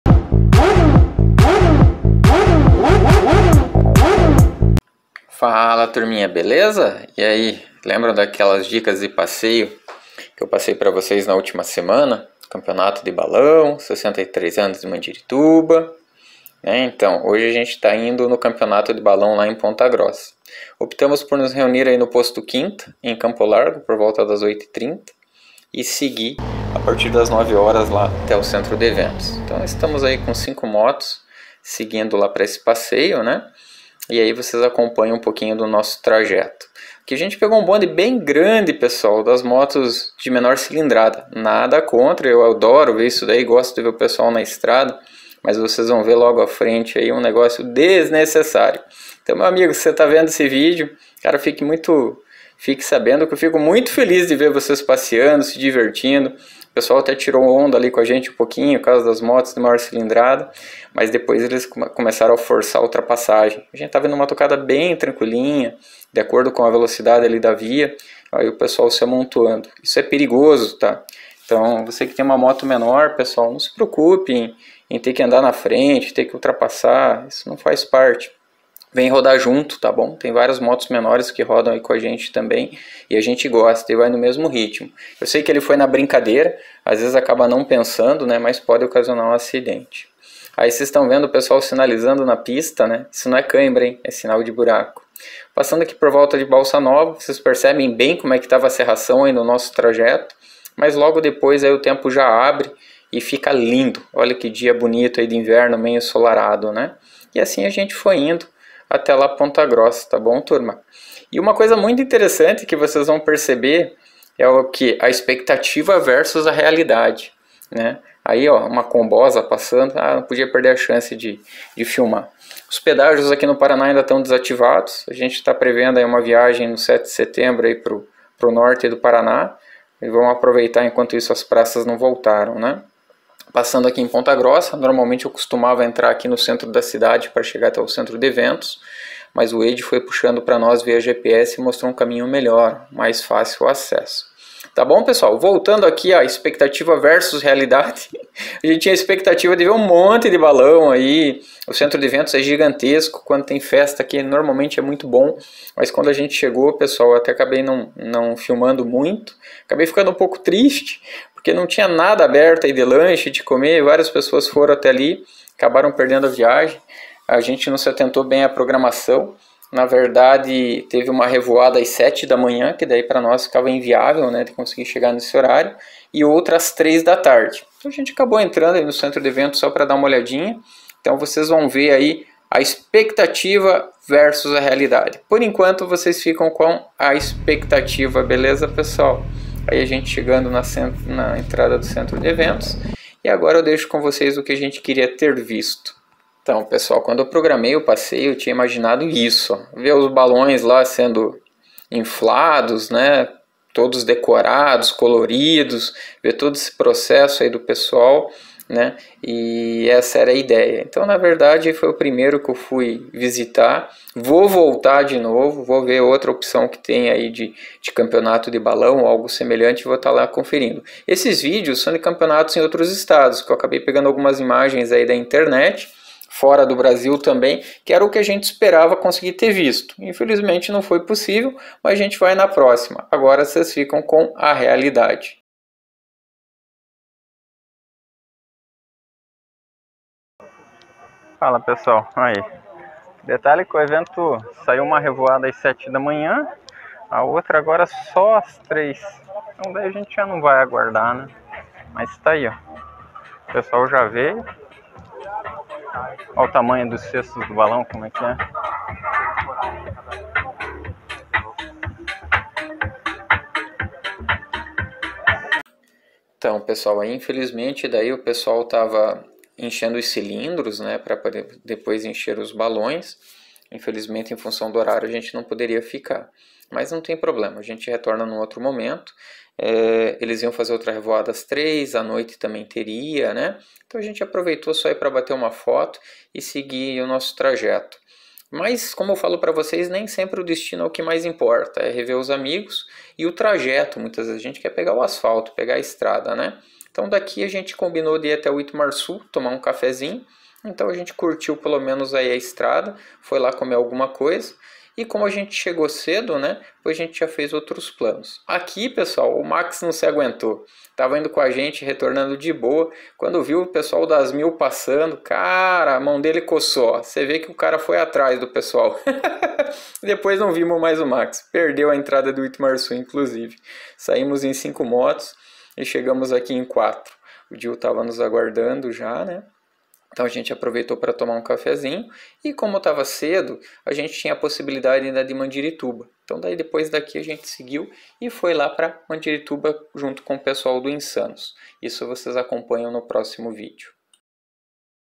Primeiro Campeonato de Balão Ponta Grossa Balonismo 2023 Centro de Eventos
Expectativa Vs. Realidade - Nem sempre o destino é como imaginamos, algumas vezes por falha no nosso planejamento, mas ainda assim não significa que o dia não foi bom.. Pelo contrário, foi ótimo pegar a estrada e estar na compainha de amigos. No próximo estaremos lá.

Como não pudemos permanecer no evento, vou deixar o link de um canal que gravou um pouquinho do evento para quem tiver curiosidade em saber o que perdemos.. rs